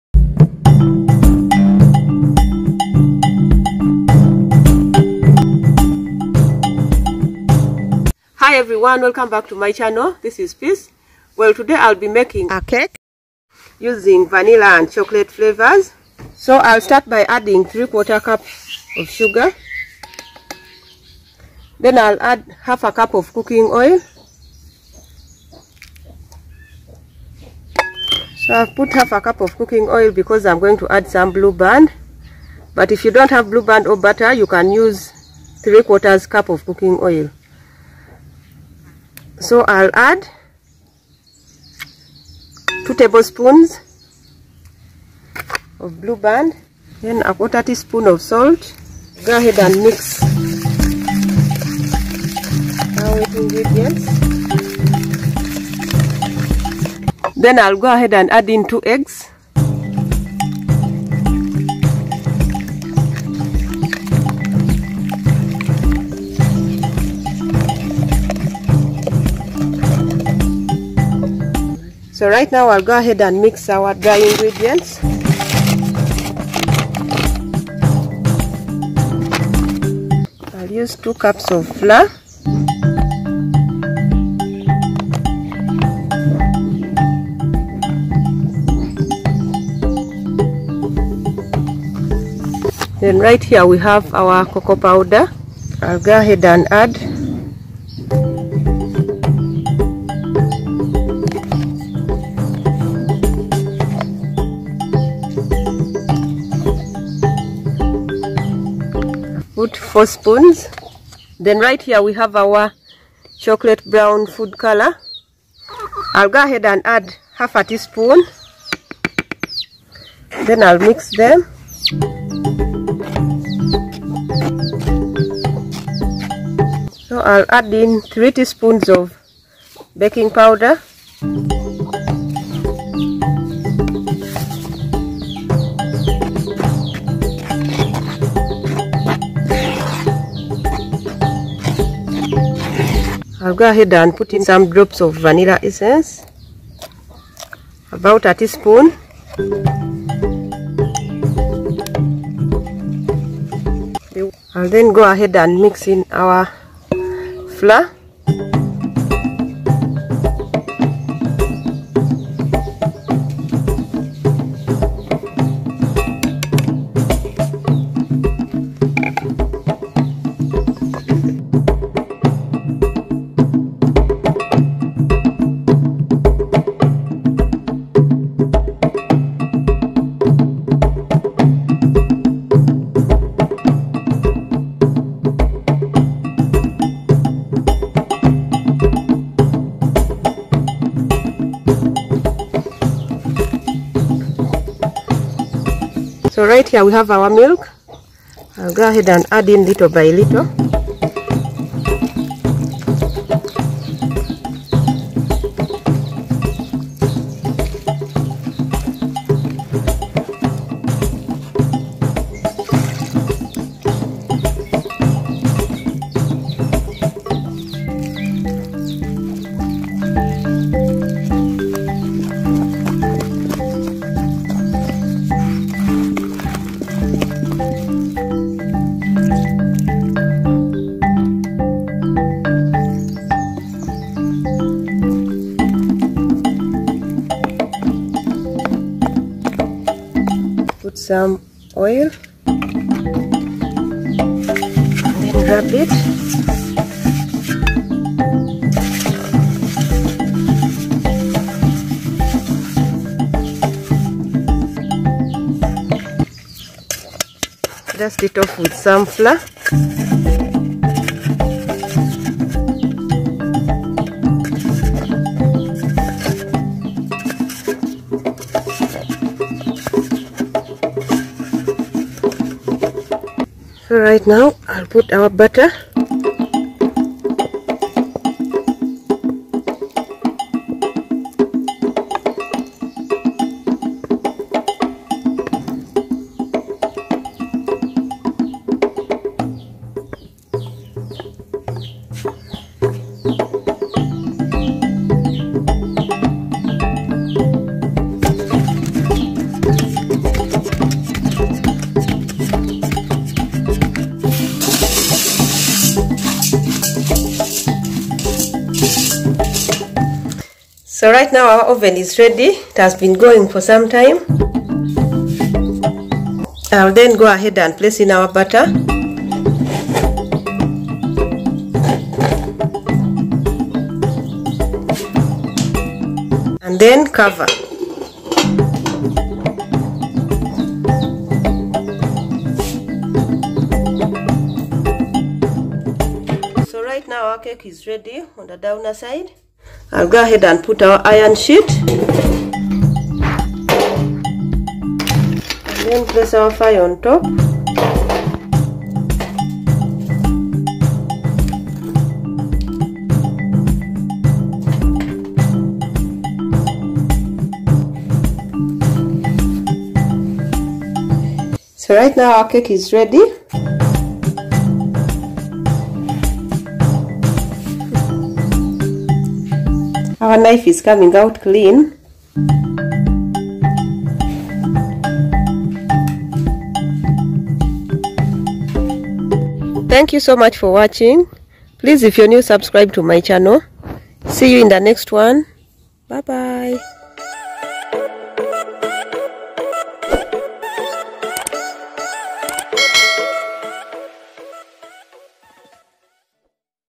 How to make a chocolate vanilla cake using a charcoal stove//African village life.nooven cake.
chocolate cakes are very delicious and easy to make